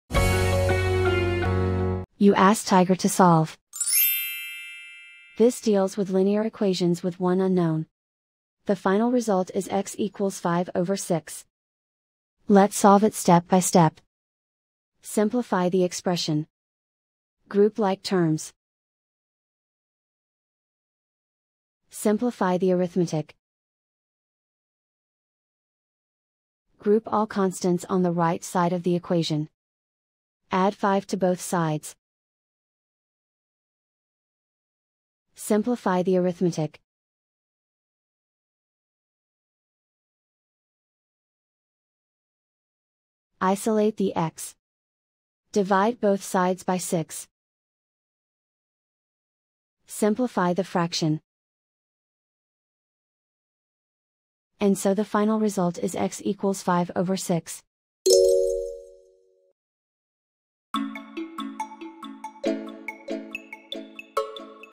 Solve 3x-2+x+2x-3=0: Linear Equation Video Solution | Tiger Algebra
Struggling with 3x-2+x+2x-3=0? Watch our step-by-step video on Tiger Algebra to master this linear equation effortlessly. Also check out for more tips and tricks for dealing with linear equations with one unknown